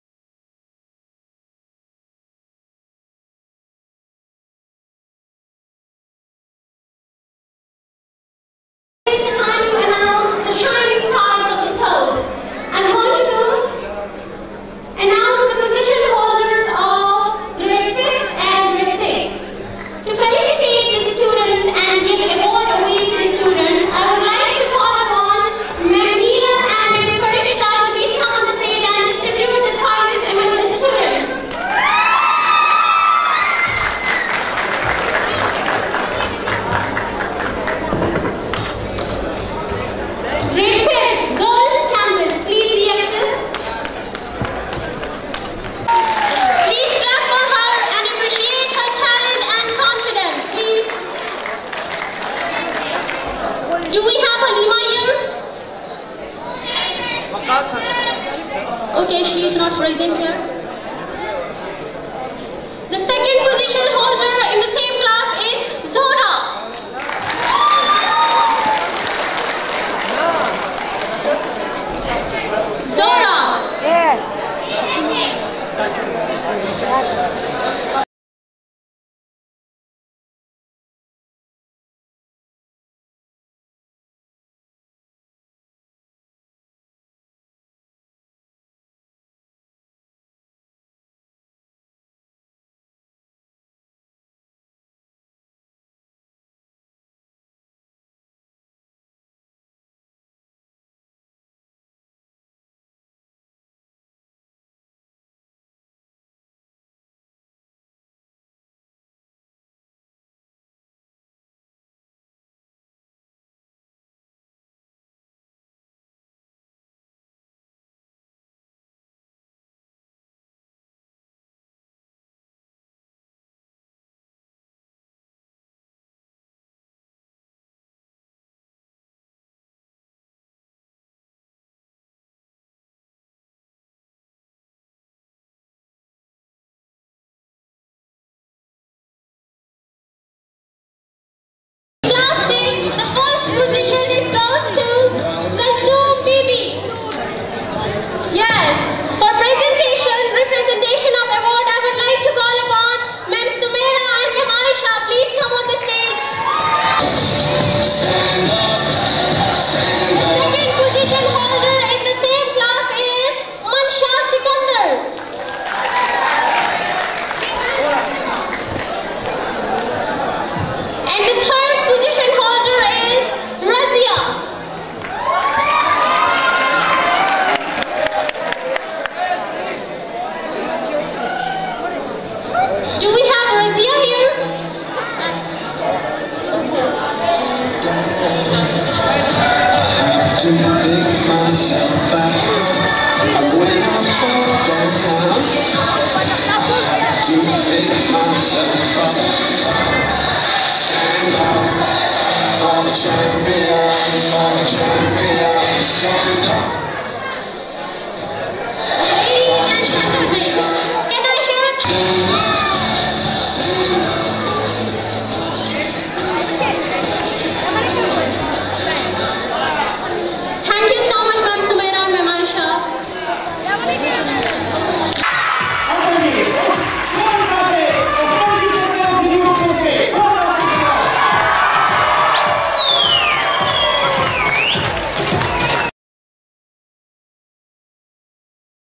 GIRLS CAMPUS POSITION HOLDERS IN THE ANNUAL AWARD CEREMONY AT NISHTAR HALL PESHAWAR 26 FEB 2023
Annual Prize distribution ceremony of Girls Campus for the session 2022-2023 in nashtar hall .